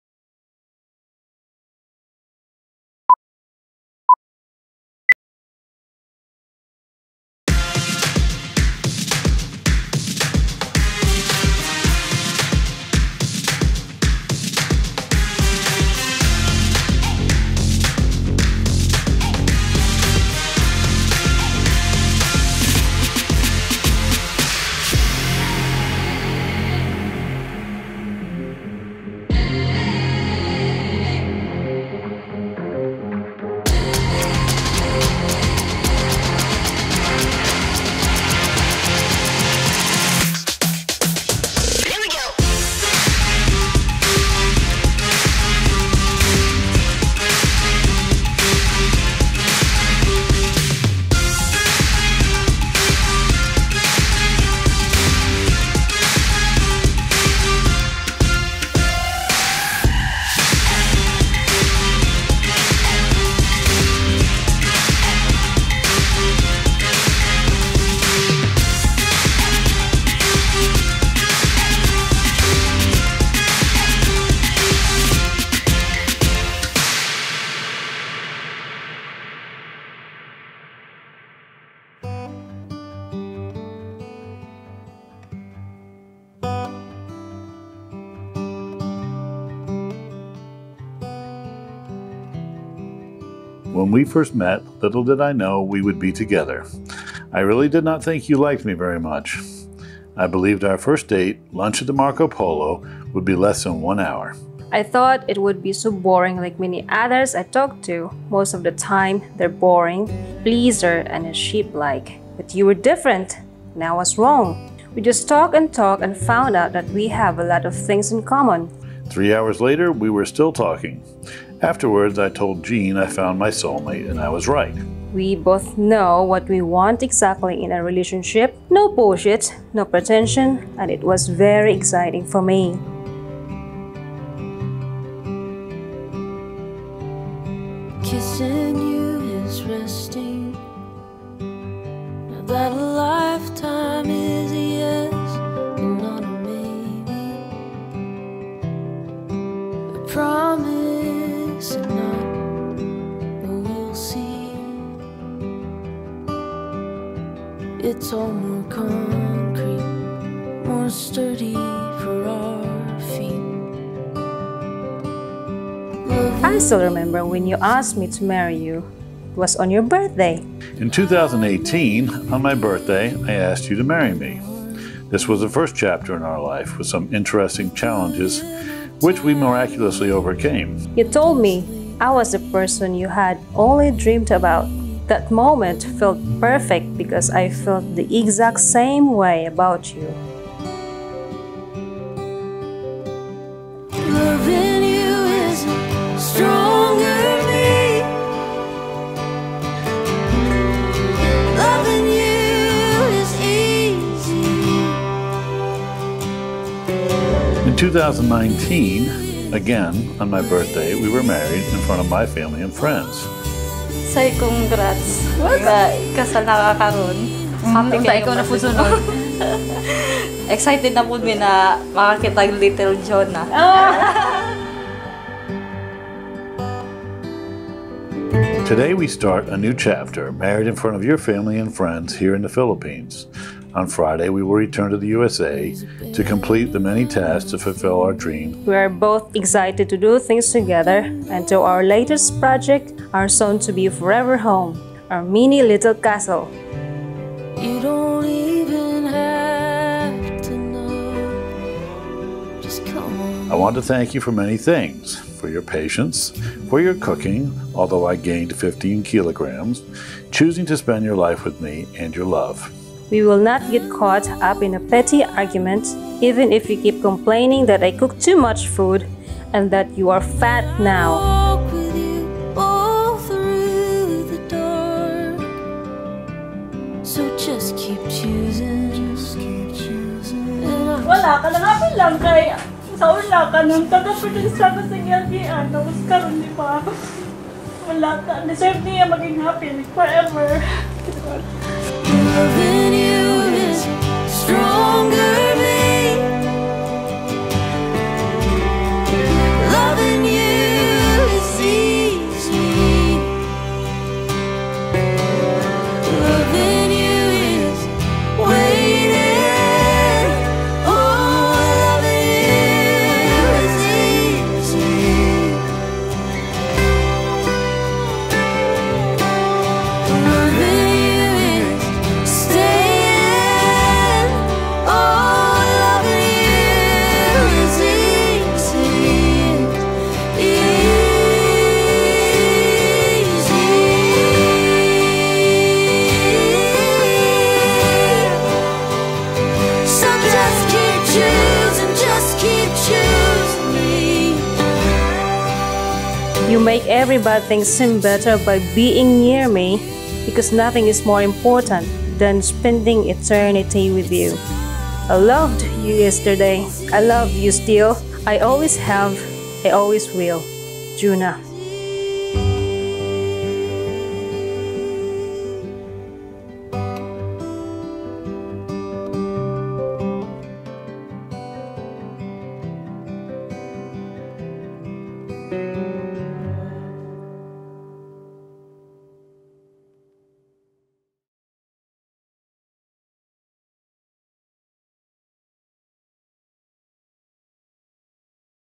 The wedding in Davao Philippines | Jhuna & Benjamin
Happy is the man who finds a true friend, and far happier is he who finds that true friend in his wife.

Congratulations Jhuna & Benjamin Markham for the wonderful celebration here in the Philippines. May your union be fruitful and a joyous one. Creative Kings Event Supplier

The Wedding of Jhuna & Ben